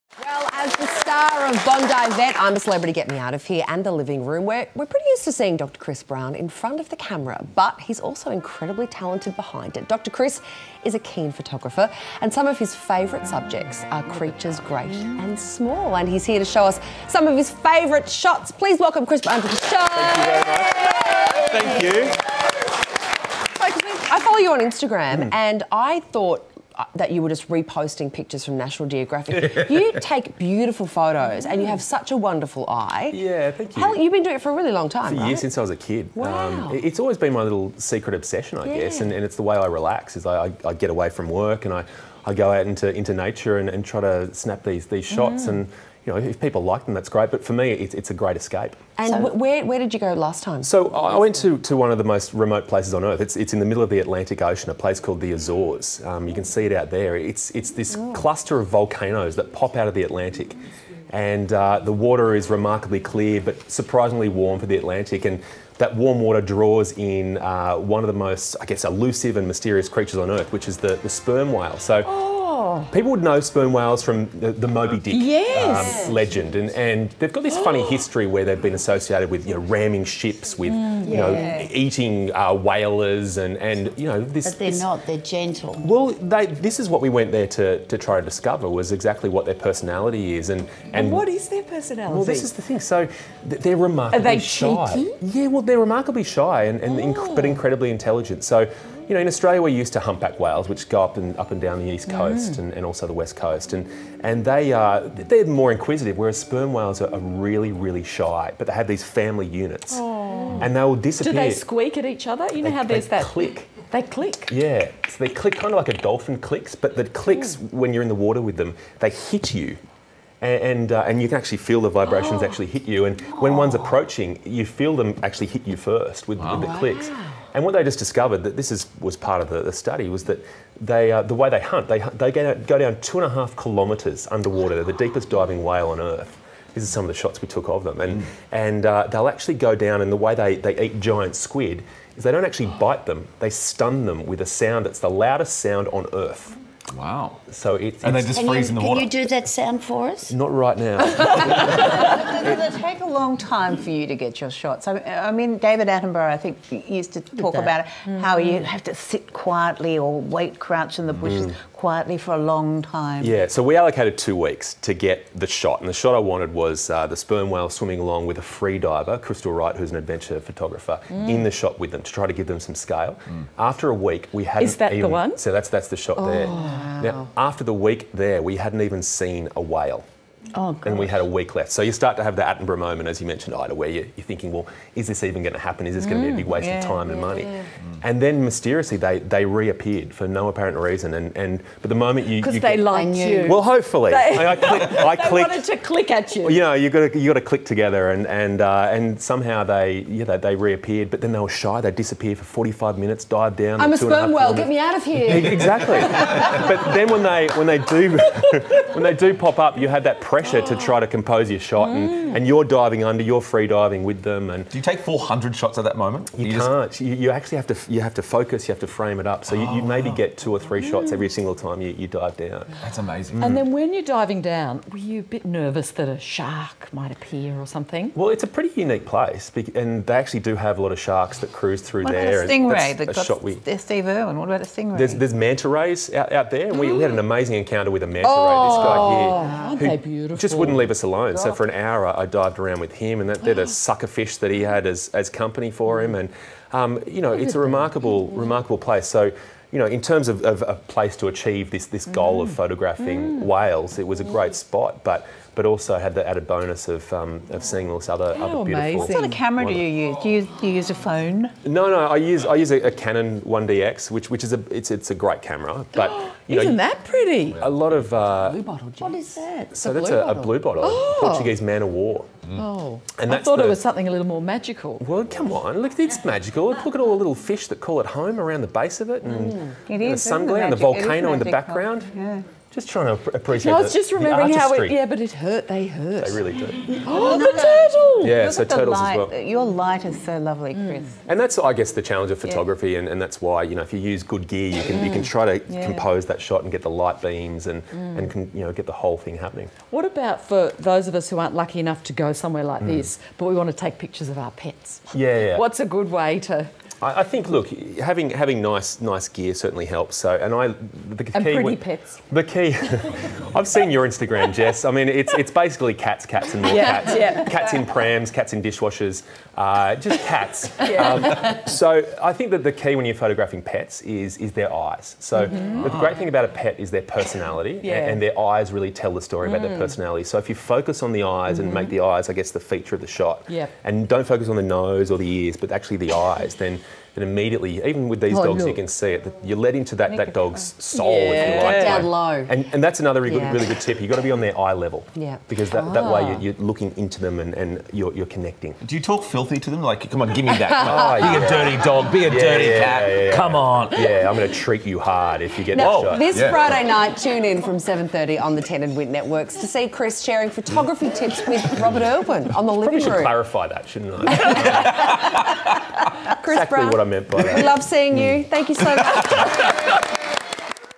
Dr Chris Brown's Photography
We're used to seeing Dr Chris Brown in front of the camera, but he's also a very keen photographer. He shares some of his breathtaking shots of wildlife.